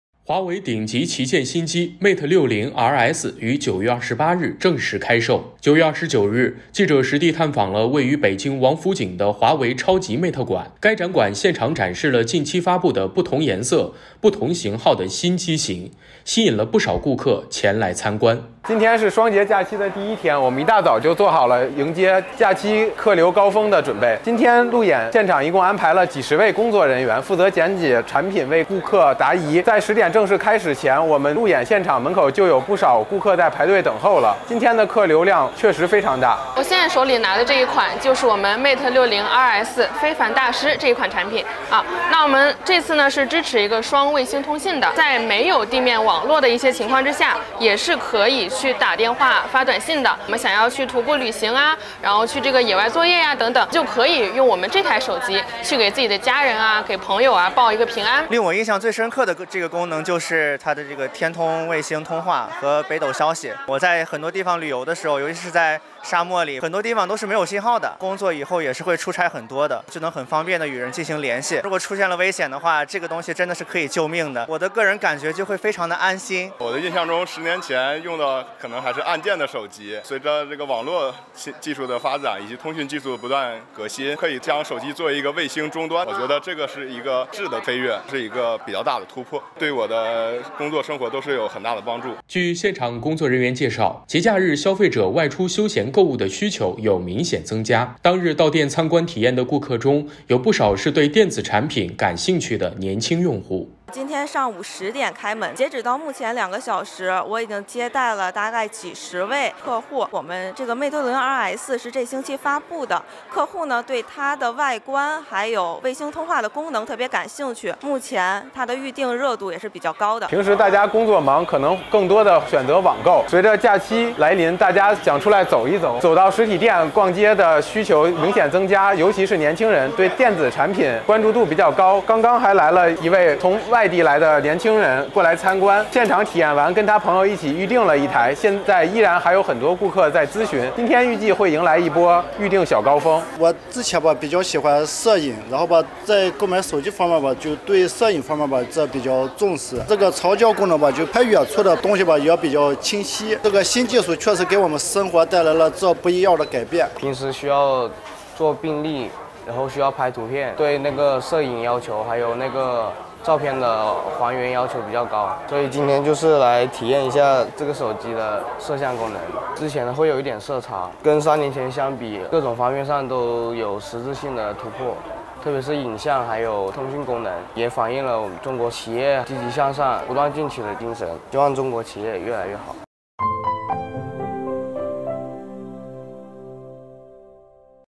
华为新机型Mate 60 RS发售：实体店“双节”期间迎客流高峰
华为顶级旗舰新机Mate 60 RS于9月28日正式开售。9月29日，记者实地探访了位于北京王府井的华为超级Mate馆，该展馆现场展示了近期发布的不同颜色不同型号的新机型，吸引了不少顾客前来参观。

据现场工作人员介绍，节假日消费者外出休闲购物的需求有明显增加，当日到店参观体验的顾客中，有不少是对电子产品感兴趣的年轻用户。